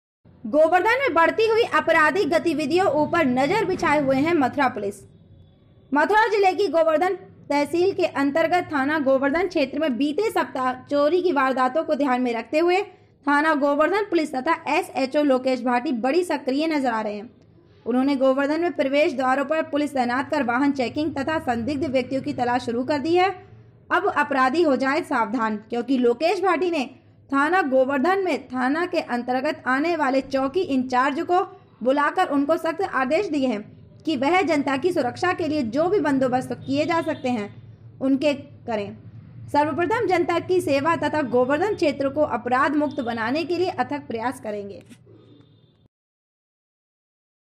गोवर्धन में बढ़ती हुई अपराधिक गतिविधियों ऊपर नजर बिछाए हुए मथुरा पुलिस
मथुरा जिले की गोवर्धन तहसील के अंतर्गत थाना गोवर्धन  क्षेत्र में बीते   सप्ताह चोरी की वारदातों को ध्यान में रखते हुए थाना गोवर्धन पुलिस तथा  एसएचओ  लोकेश भाटी बड़ी सक्रिय नजर आ रहे हैं उन्होंने गोवर्धन  मैं प्रवेश द्वारों पर पुलिस तैनात कर वाहन चेकिंग तथा संदिग्ध व्यक्तियों की तलाशी शुरू कर दी है अब अपराधी हो जाएं सावधान  क्योंकि लोकेश भाटी  ने खाना गोवर्धन में थाना के अंतर्गत आने  वाले चौकी इंचार्ज को बुलाकर उनको सख्त आदेश दिए हैं कि वह जनता की सुरक्षा के लिए जो भी बंदोबस्त किए जा सकते हैं उनको करें सर्वप्रथम जनता की सेवा तथा गोवर्धन  क्षेत्र को  अपराध मुक्त बनाने के लिए अथक प्रयास करेंगे